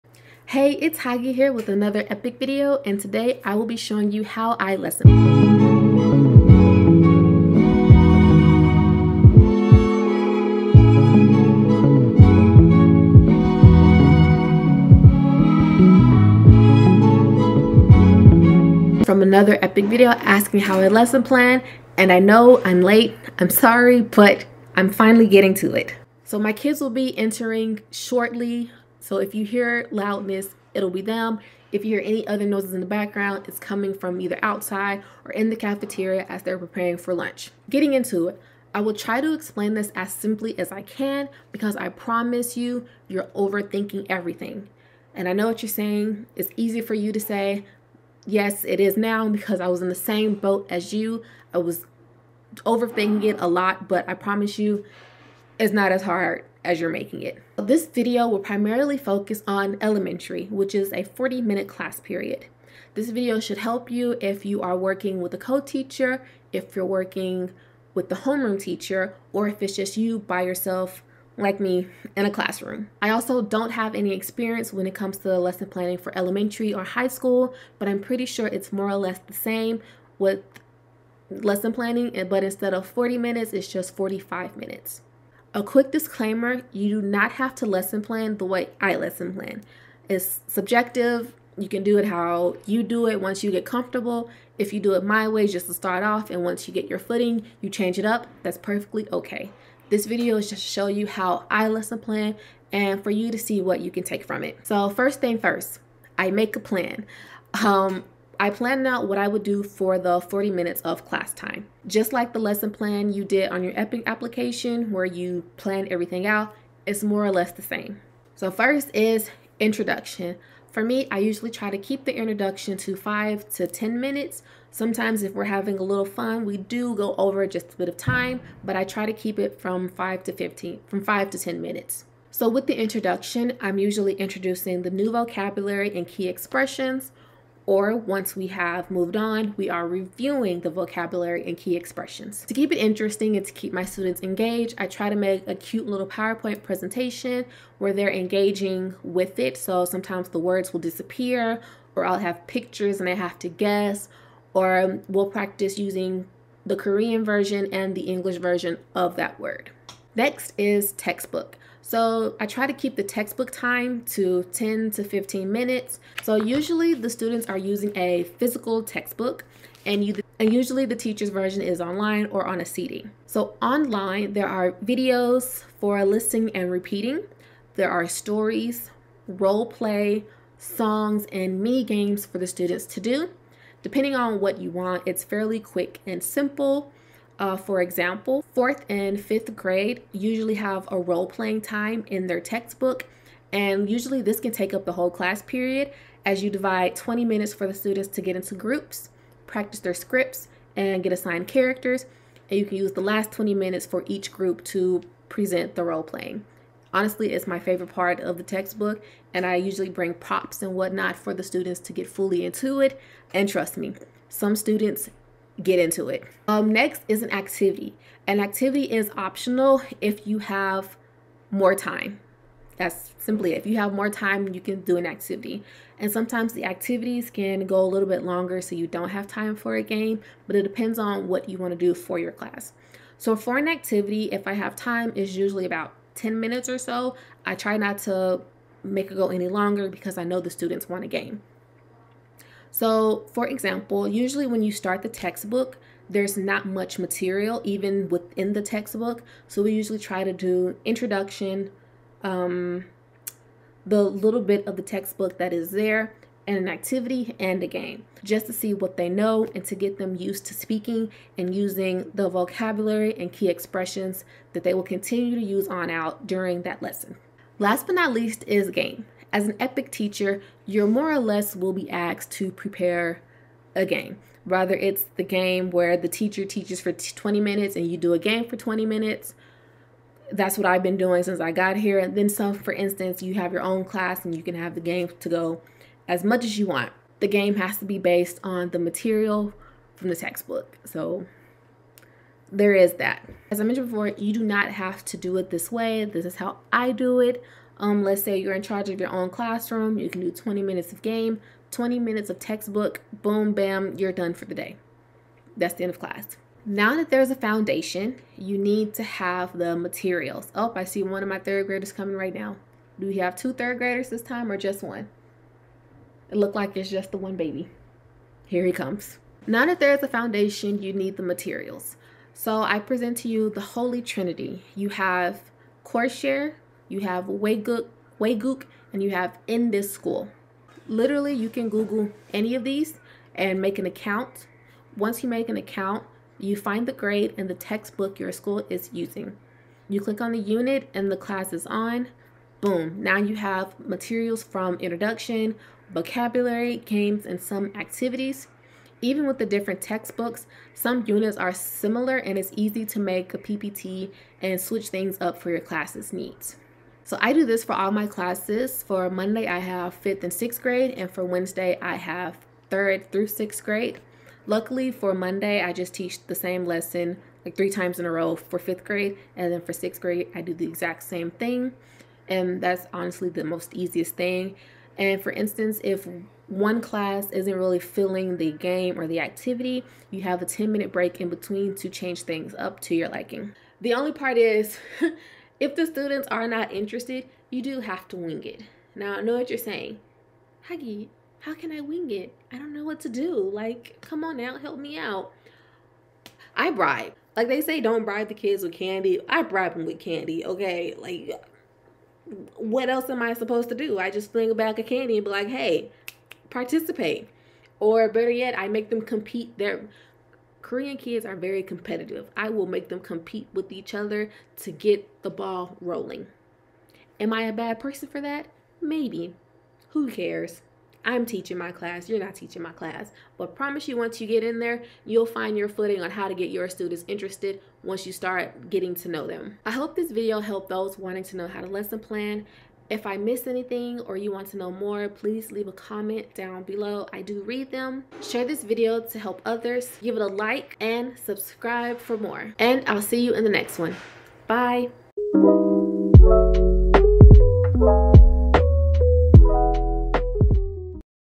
TEACHING IN KOREA : Lesson Planning 📝 How an EPIK Teacher does it
Hello! Everyone, it's Haggy here with another EPIK video where I show you guys how I lesson plan for my classes! Be sure to Share to help out the new teachers!

Skip Here

Intro - 1:59
Textbook - 2:40
Activities - 3:37
Game - 4:31
Materials - 5:25

If I missed anything or you want to know more, leave a comment for others looking for help!

Here are some websites we use for planning some activities, please note that some of them need to be paid for, but they’re super useful, especially Koreshare.

1. Koreshare

2. TaysTeachingtoolkit

3. Waygook

4. Baamboozle

5. ESL games plus

6. Games4ESL

7. Twinkl

8. Kahoot

You are welcome to ask any questions should you have any

it’s haggy here and I'm currently an English teacher here in Korea.
Enjoy my videos as this is basically a diary.